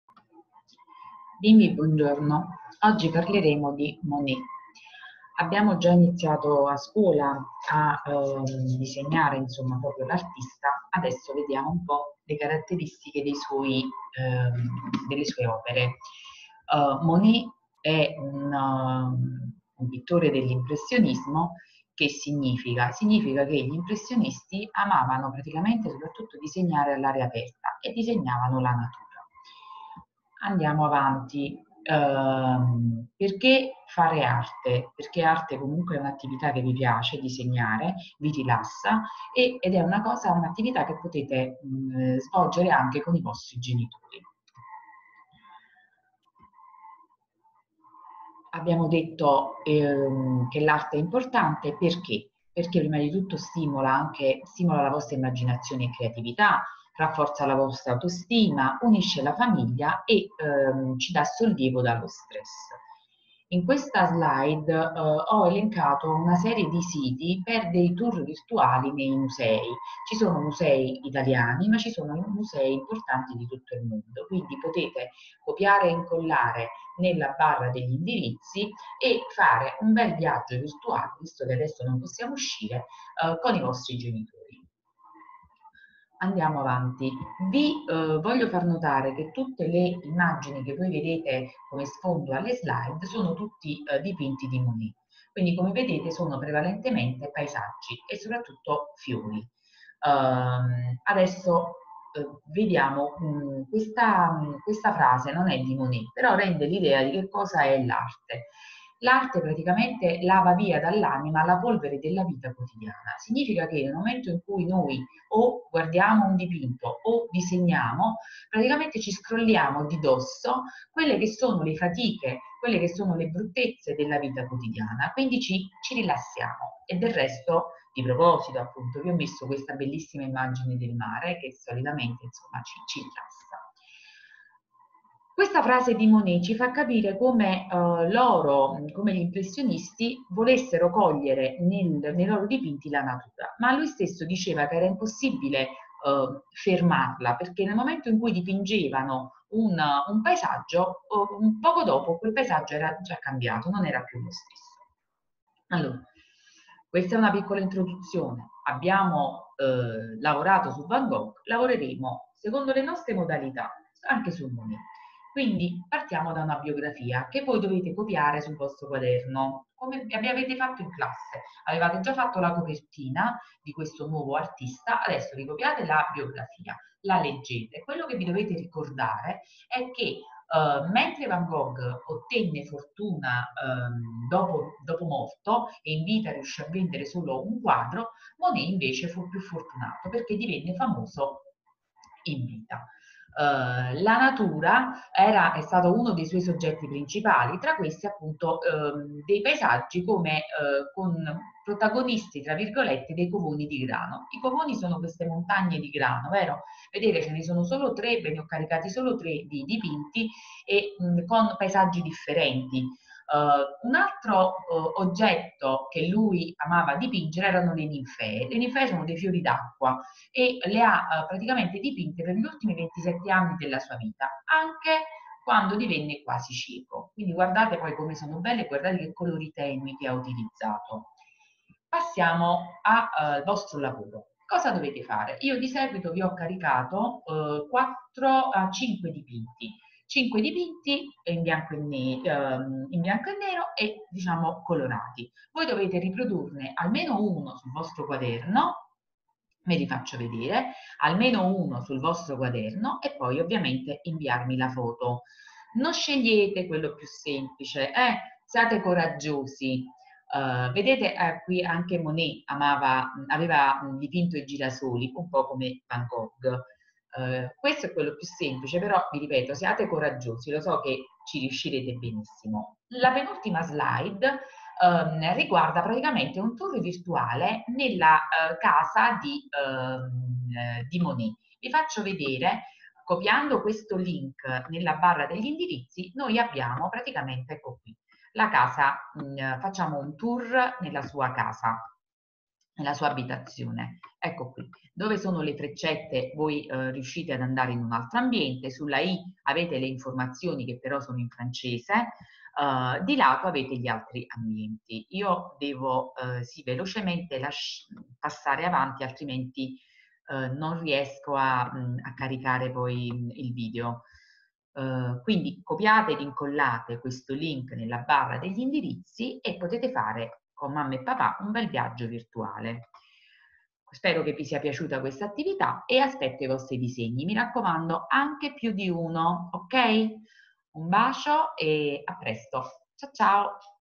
Monet, il benessere del disegno e dell'arte
Didattica a distanza per le classi IV, Scuola Primaria